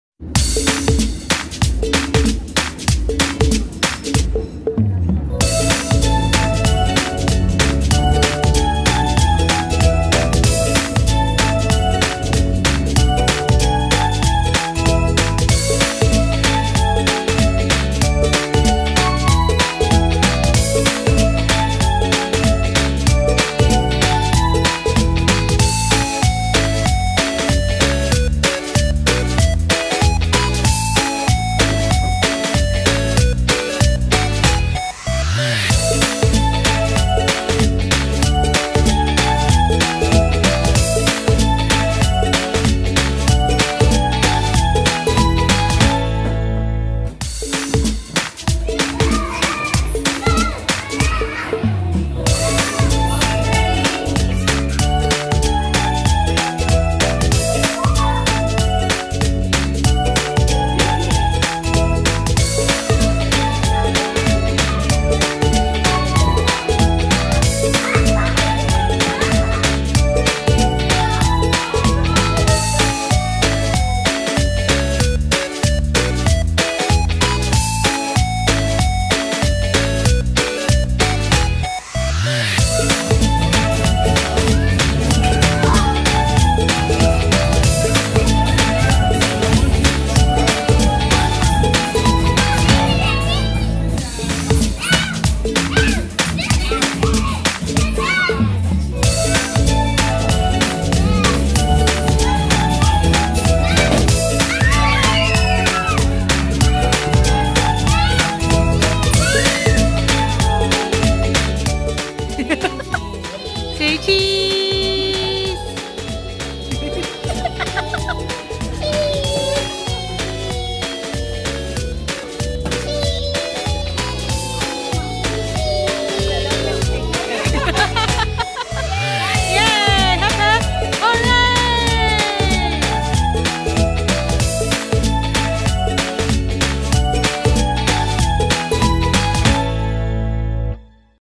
Attending 4th birthday party | MummyDee & LilDaniel adventures
Alessio's 4th birthday party at Nuffield Health Centre.